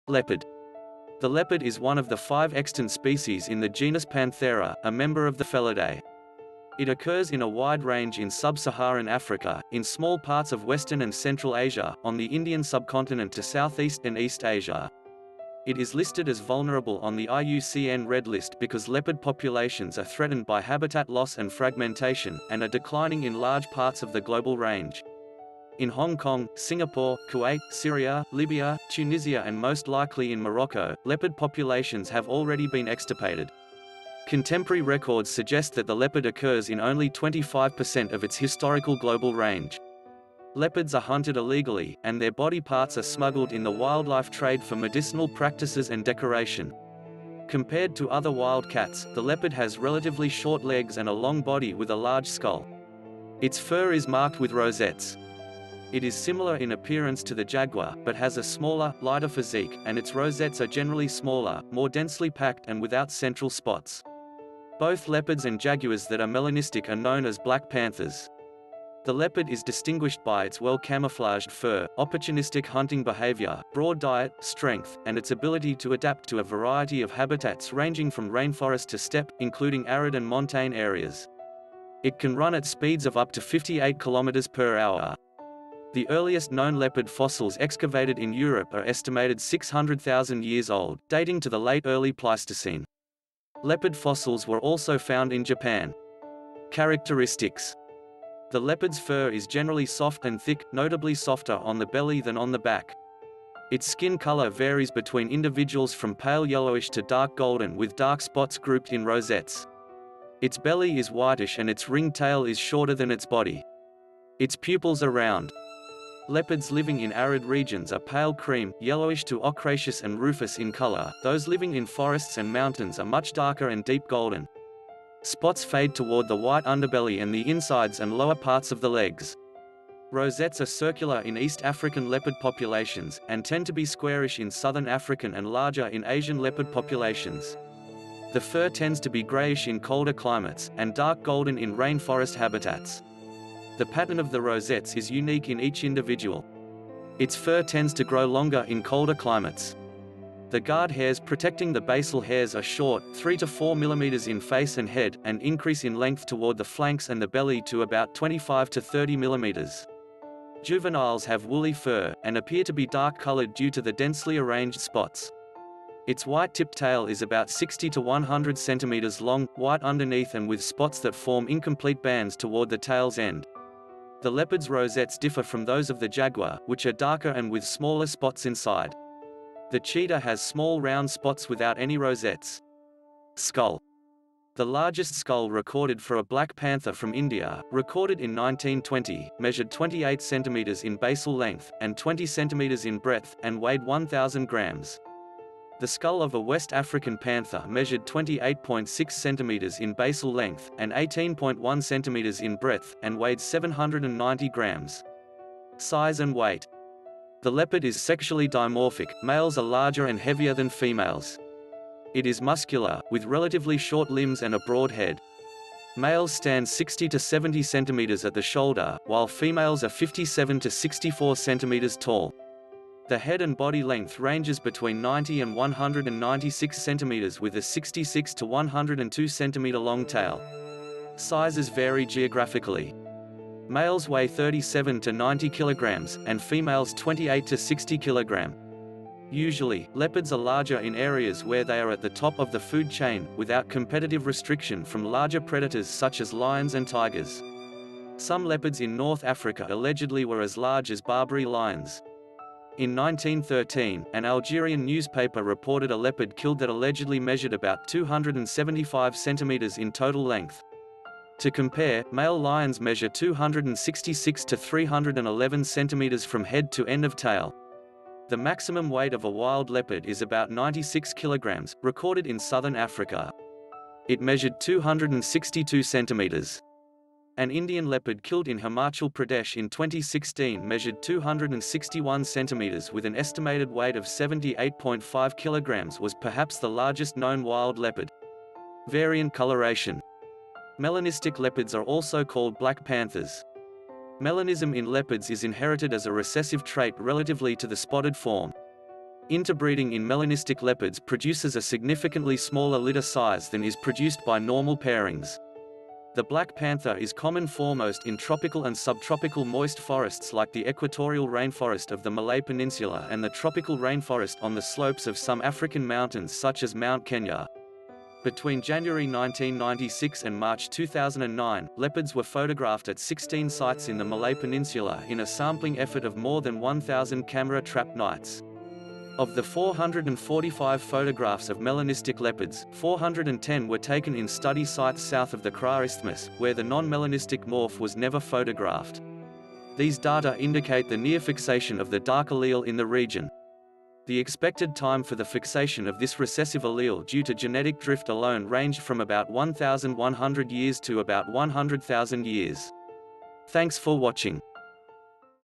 Leopard || Description, Characteristics and Facts!
The leopard (Panthera pardus) is one of the five extant species in the genus Panthera, a member of the Felidae. It occurs in a wide range in sub-Saharan Africa, in small parts of Western and Central Asia, on the Indian subcontinent to Southeast and East Asia. It is listed as Vulnerable on the IUCN Red List because leopard populations are threatened by habitat loss and fragmentation, and are declining in large parts of the global range. In Hong Kong, Singapore, Kuwait, Syria, Libya, Tunisia and most likely in Morocco, leopard populations have already been extirpated. Contemporary records suggest that the leopard occurs in only 25% of its historical global range

Leopards are hunted illegally, and their body parts are smuggled in the wildlife trade for medicinal practices and decoration.

Compared to other wild cats, the leopard has relatively short legs and a long body with a large skull. Its fur is marked with rosettes. It is similar in appearance to the jaguar, but has a smaller, lighter physique, and its rosettes are generally smaller, more densely packed and without central spots. Both leopards and jaguars that are melanistic are known as black panthers. The leopard is distinguished by its well-camouflaged fur, opportunistic hunting behaviour, broad diet, strength, and its ability to adapt to a variety of habitats ranging from rainforest to steppe, including arid and montane areas. It can run at speeds of up to 58 kilometres per hour. The earliest known leopard fossils excavated in Europe are estimated 600,000 years old, dating to the late Early Pleistocene. Leopard fossils were also found in Japan.